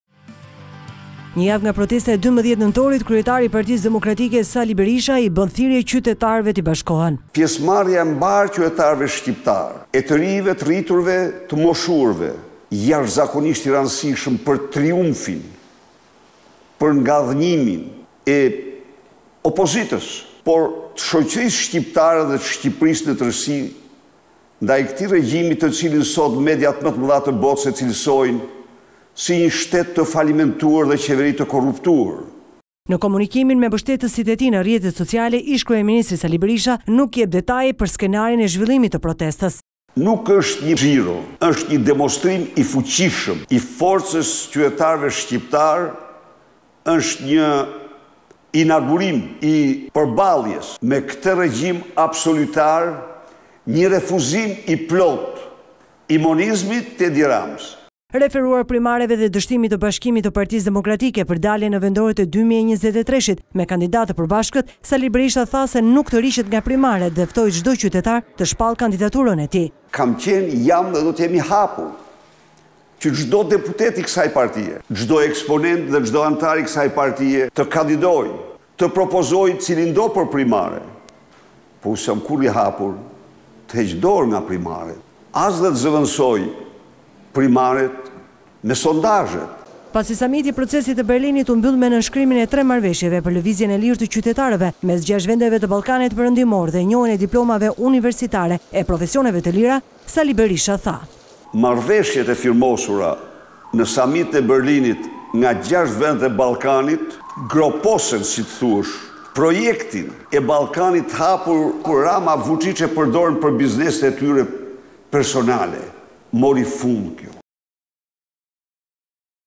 Protesta e 12 nëntorit/ Berisha: Do të jetë një kthesë e madhe -
Kreu i PD-së, Sali Berisha gjatë një bashkëbisedimi me qytetarët në rrjete sociale, ka folur për protestën e 12 nëntorit.Sipas tij, ndodhemi në një proces ku çdo njeri prej nesh duhet të marrë përgjegjësi të bëjë çdo gjë për një ftesë të rëndësishme të kësaj shoqërie dhe këtij vendi “Ndodhemi në një proces ku çdo njeri prej nesh duhet të marrë përgjegjësi të bëjë çdo gjë për një ftesë të rëndësishme të kësaj shoqërie dhe këtij vendi. PD është pikërisht e përqendruar në këtë kthesë në realizimin e kësaj kthese të madhe. ndaj zgjodha të flasë me ju për axhendën tonë, që konsiston në protestën e fuqishme të orës 16:00 të datës 12 nëntor në të cilën pjesëmarrja juaj, e mbarë qytetarëve shqiptarë, e të rinjve, të moshuarve, është një kusht me rëndësi për triumf.”, tha Berisha.